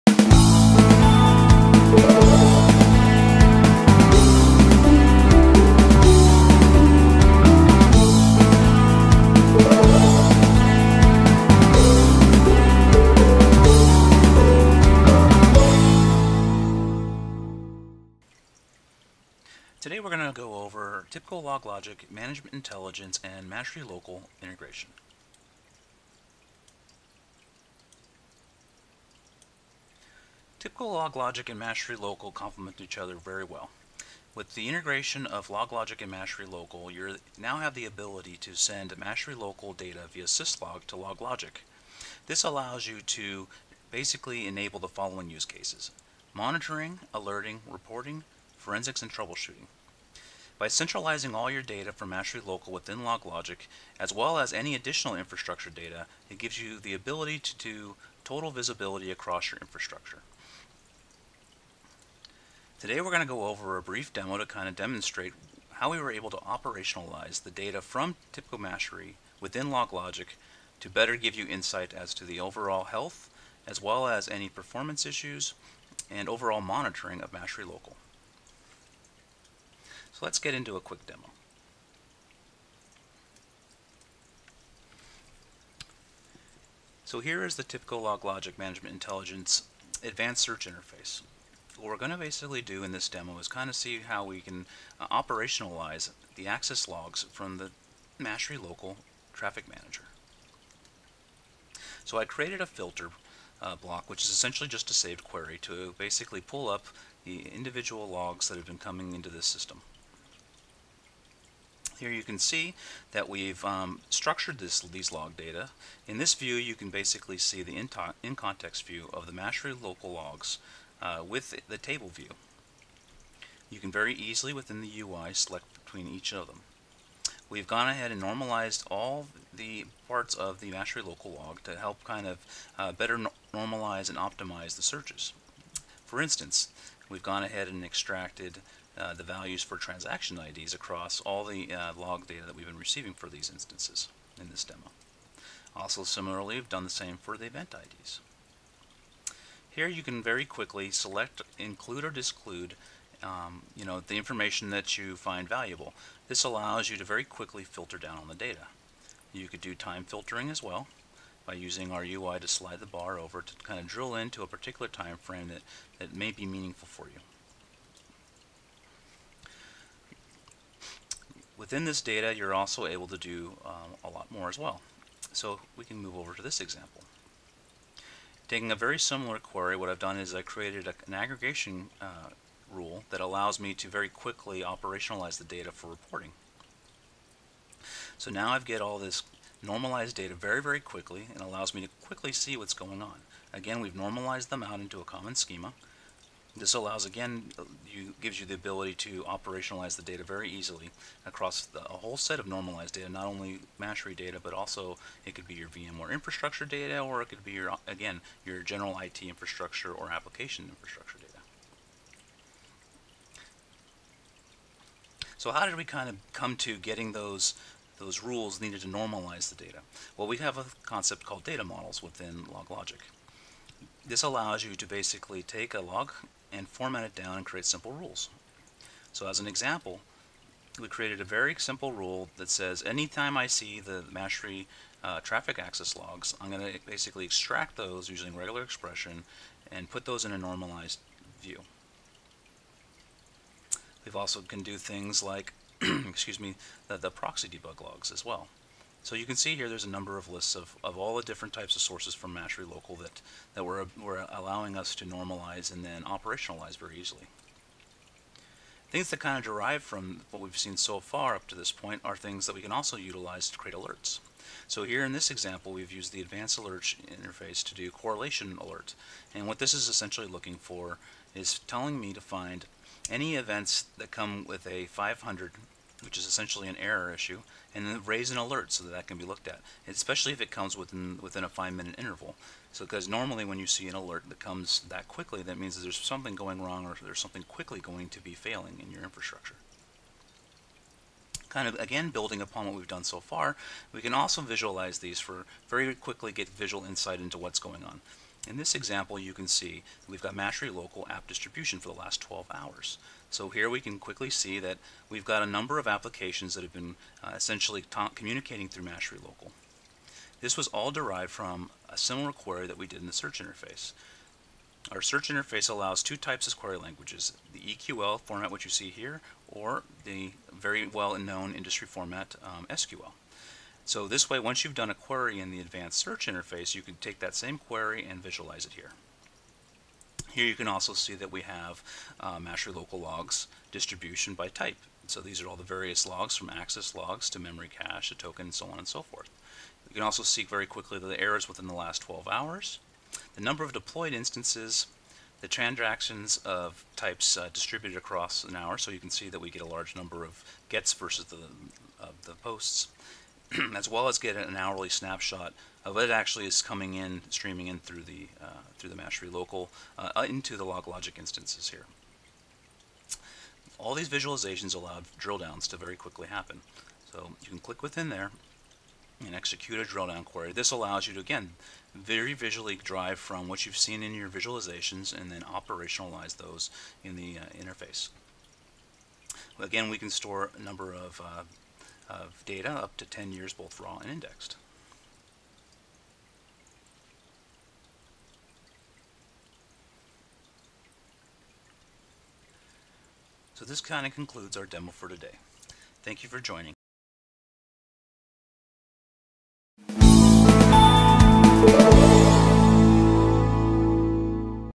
How to Monitor TIBCO Mashery Local using TIBCO LogLogic
Learn how you can use TIBCO LogLogic to consume Mashery Local access logs to monitor your Mashery local environment.